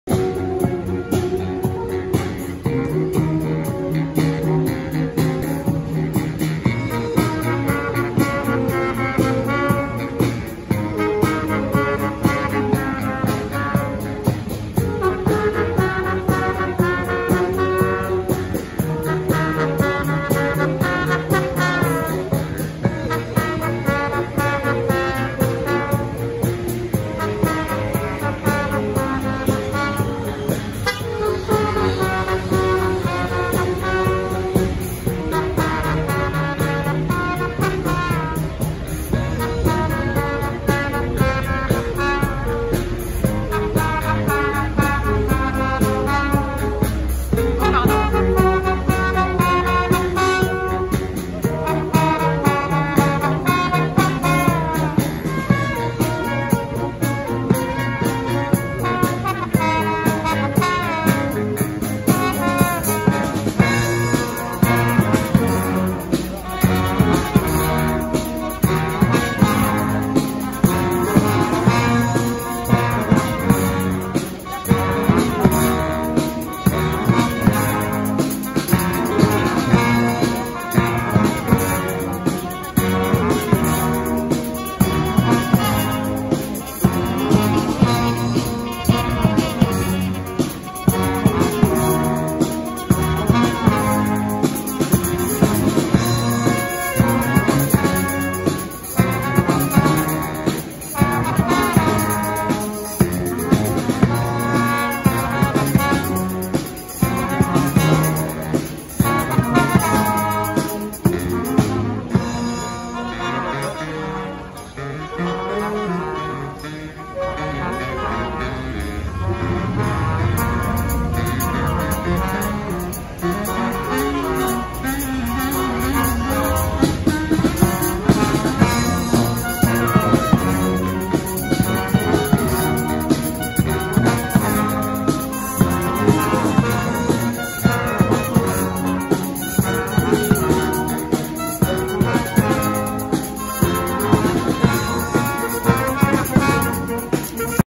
Daft Punk gets a fanfare makeover: Incredible student performance
I spent an nice afternoon at the Place des Jacobins in Lyon  watching these students performing in the street.